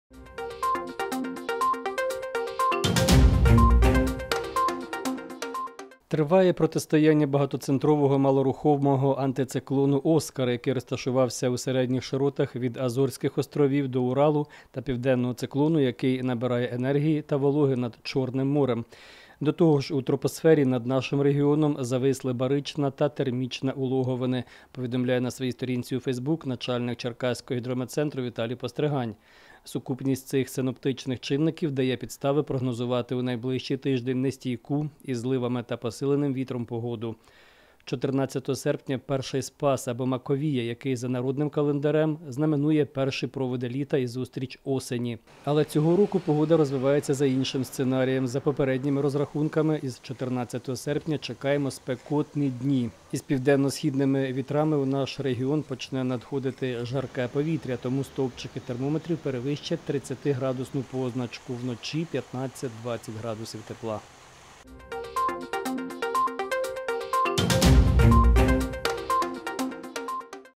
Припече: на медовий Спас на Черкащині стовпчики термометрів перевищать 30° позначку
Триває протистояння багатоцентрового малорухомого антициклону «OSKAR», який розташувався у середніх широтах від Азорських островів до Уралу та південного циклону, який набирає енергії та вологи над Чорним морем. До того ж у тропосфері над нашим регіоном «зависли» барична та термічна улоговини, повідомляє на своїй сторінці у Фейсбук начальник Черкаського гідрометцентру Віталій Постригань.

Сукупність цих синоптичних чинників дає підстави прогнозувати у найближчий тиждень нестійку, із зливами та посиленим вітром погоду. 14 серпня – «Перший Спас» або «Маковія», який за Народним календарем знаменує перші проводи літа і зустріч осені. Але цього року погода розвивається за іншим сценарієм.

За попередніми розрахунками, із 14 серпня, чекаємо спекотні дні. Із південно-східними вітрами у наш регіон почне надходити жарке повітря, тому стовпчики термометрів перевищать 30° позначку, вночі 15-20º.